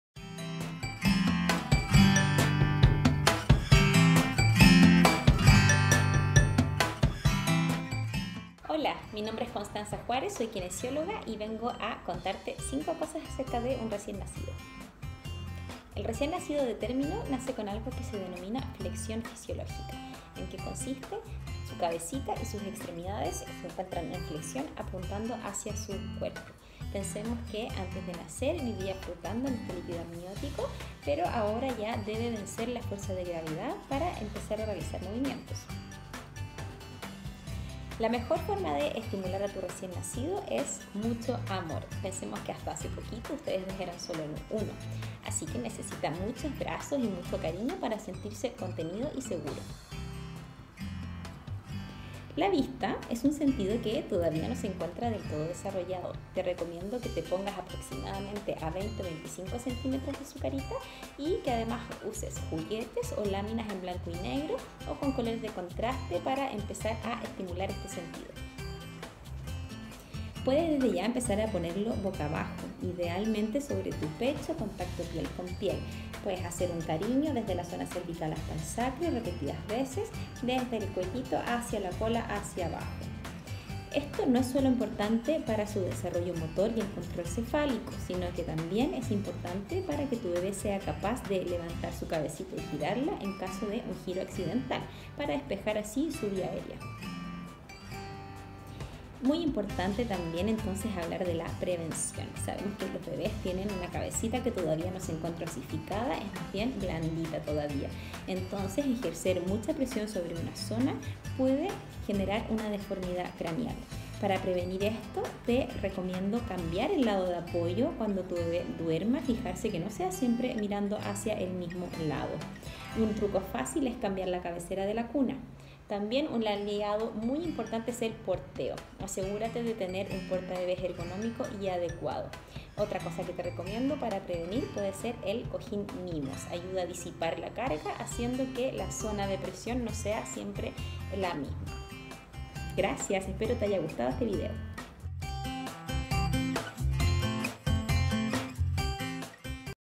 5 tips que debes saber con un recien nacido
Constanza Juarez, Kinesióloga, nos entrega detalles a considerar para cuando  llegue tu bebé